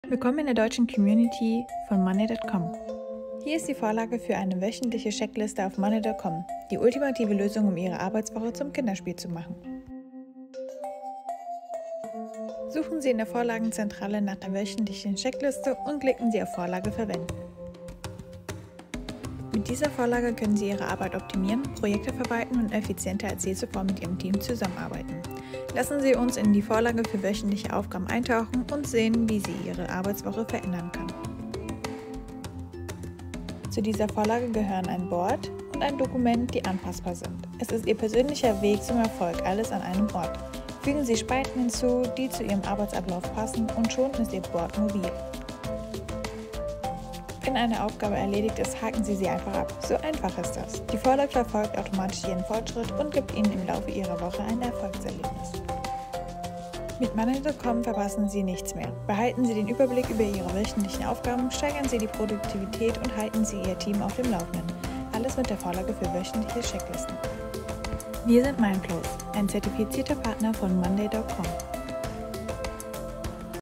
Monday.com Tutorial | Wöchentlich Checkliste
In diesem Video erfahren Sie mehr über die wöchentliche Checklistenvorlage auf Monday.com:

- Verwenden Sie ein gemeinsames Monday-Arbeitsdokument

- Sammeln Sie die wöchentliche Arbeit Ihres Teams

📌 Passt Monday.com zu Ihnen? Buchen Sie hier einen KOSTENLOSEN Anruf bei uns